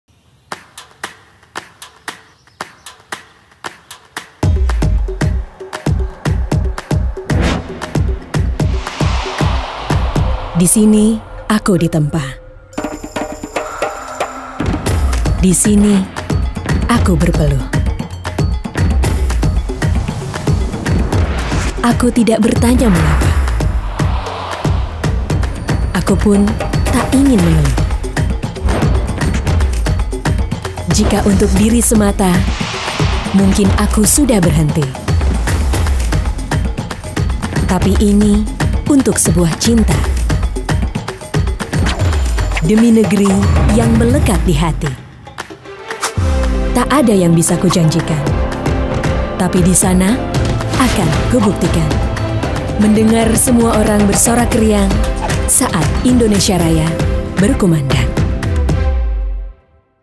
PSA WUSHU for Asian Games 2018
Wushu Campaign with its ambassador. world champion - Linsdwell Kwok.

Film Director & Creative Director : Iman Brotoseno
DOP : Roy Lolang
Producer : Moming
PH : Orange Waterland